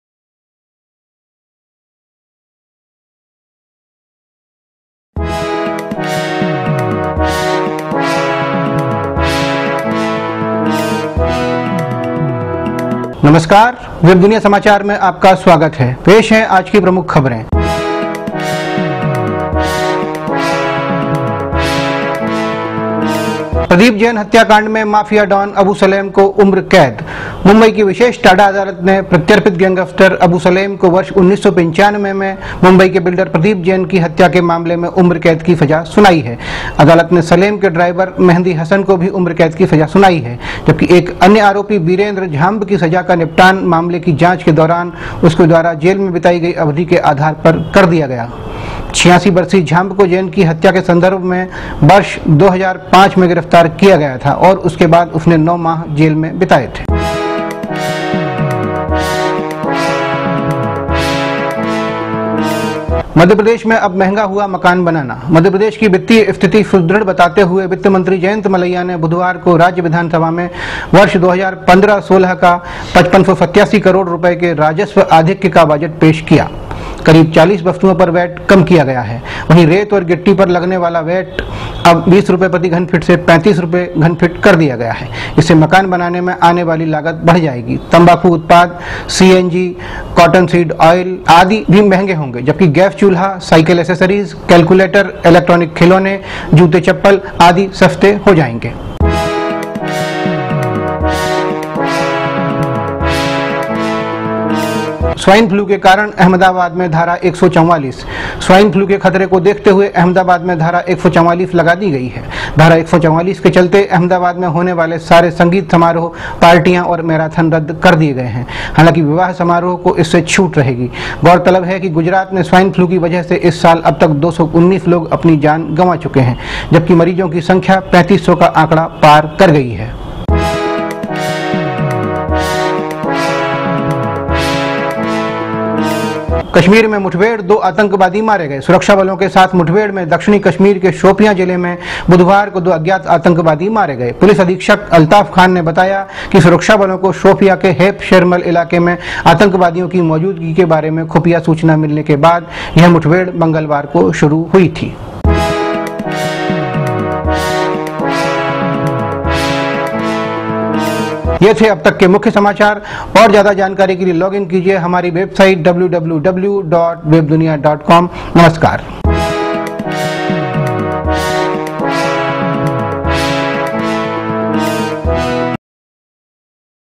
माफिया डॉन अबू सलेम को उम्रकैद
मुंबई की विशेष टाडा अदालत ने प्रत्यर्पित गैंगस्टर अबू सलेम को वर्ष 1995 में मुंबई के बिल्डर प्रदीप जैन की हत्या के मामले में उम्रकैद की सजा सुनाई है। अदालत ने सलेम के ड्राइवर मेहंदी हसन को भी उम्रकैद की सजा सुनाई है, जबकि एक अन्य आरोपी वीरेंद्र झांब की सजा का निपटान मामले की जांच के दौरान उसके द्वारा जेल में बिताई अवधि के आधार पर कर दिया गया।